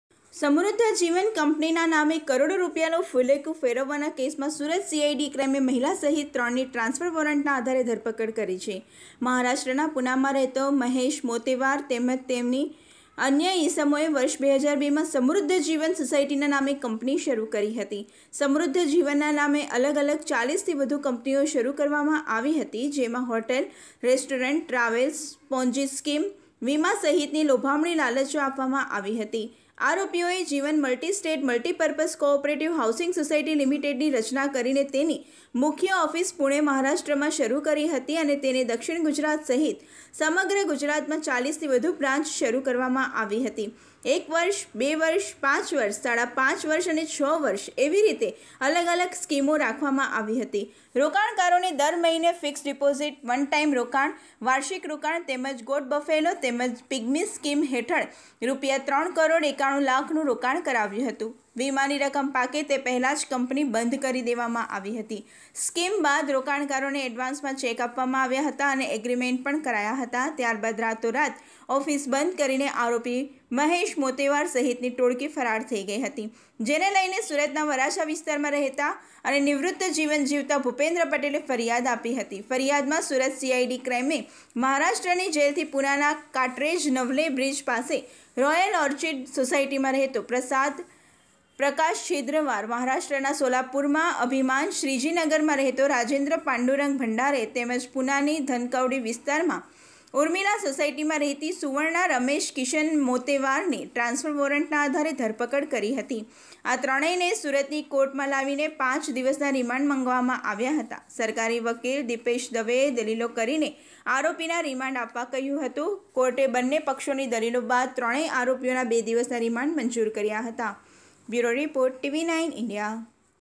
સુરતમાં સમૃદ્ધ જીવન કંપનીના નામે કરોડો રૂપિયાનું ફૂલેકું ફેરવવાના કેસમાં મહિલ...|TV9INDIA GUJARATI
મહારાષ્ટ્રના પુનામાં રહેતો મહેશ મોતેવાર તેમજ તેની અન્ય ઇસમોએ વર્ષ-2002માં સમૃદ્ધ જીવન સોસાયટીના નામે કંપની શરૂ કરી હતી. સમૃદ્ધ જીવનના નામે અલગ અલગ 40થી વધુ કંપનીઓ શરૂ કરવામાં આવી હતી. જેમાં હોટેલ, રેસ્ટોરન્ટ, ટ્રાવેલ્સ, પોન્જી સ્કીમ, વીમા સહિતની લોભામણી લાલચો આપવામાં આવી હતી. આરોપીઓએ જીવન મલ્ટી સ્ટેટ મલ્ટી પર્પઝ કો.ઓ. હાઉસીંગ સોસાયટી લિમિટેડની રચના કરીને તેની મુખ્ય ઓફિસ પુણે મહારાષ્ટ્રમાં શરૂ કરી હતી અને તેની દક્ષિણ ગુજરાત સહિત સમગ્ર ગુજરાતમાં 40થી વધુ બ્રાન્ચો શરૂ કરવામાં આવી હતી.એક વર્ષ, બે વર્ષ, પાંચ વર્ષ, સાડા પાંચ વર્ષ અને છ વર્ષ એવી રીતે અલગ અલગ સ્કીમો રાખવામાં આવી હતી. રોકાણકારોને દર મહિને, ફિક્સ ડિપોઝિટ, વન ટાઇમ રોકાણ, વાર્ષિક રોકાણ તેમજ ગોટ, બફેલો તેમજ પીગ્મી સ્કીમ હેઠળ રૂા. 3.91 કરોડનું રોકાણ કરાવ્યું હતું. વીમાની રકમ પાકે તે પહેલા જ કંપની બંધ કરી દેવામાં આવી હતી. સ્કીમ બાદ રોકાણકારોને એડવાન્સમાં ચેક આપવામાં આવ્યા હતા અને એગ્રીમેન્ટ પણ કરાયા હતા. ત્યારબાદ રાતોરાત ઓફિસ બંધ કરીને આરોપી મહેશ મોતેવાર સહિતની ટોળકી ફરાર થઇ ગઇ હતી. જેને લઇને સુરતના વરાછા વિસ્તારમાં રહેતા અને નિવૃત જીવન જીવતા ભુપેન્દ્ર પટેલે ફરિયાદ કરી હતી.ફરિયાદમાં સુરત સીઆઇડી ક્રાઇમે મહારાષ્ટ્રની જેલથી પુનાના કાટરેજ નવલે બ્રિજ પાસે રોયલ ઓર્ચિડ સોસાયટીમાં રહેતો પ્રસાદ પ્રકાશ છિદ્રવાર, મહારાષ્ટ્રના સોલાપુરામાં અભીમાન શ્રીજીનગરમાં રહેતો રાજેન્દ્ર પાંડુરંગ ભંડારે તેમજ પુનાની ધનકવડી વિસ્તારમાં ઉર્મિલા સોસાયટીમાં રહેતી સુવર્ણા રમેશ કિશન મોતેવારની ટ્રાન્સફર વોરંટના આધારે ધરપકડ કરવામાં આવી હતી. આ ત્રણેયને સુરતની કોર્ટમાં લાવીને પાંચ દિવસના રિમાન્ડ માંગવામાં આવ્યા હતા. સરકારી વકીલ દિપેશ દવેએ દલીલો કરીને આરોપીના રિમાન્ડ આપવા કહ્યું હતું. કોર્ટે બંને પક્ષોની દલીલો બાદ ત્રણેય આરોપીના બે દિવસના રિમાન્ડ મંજૂર કર્યા હતા.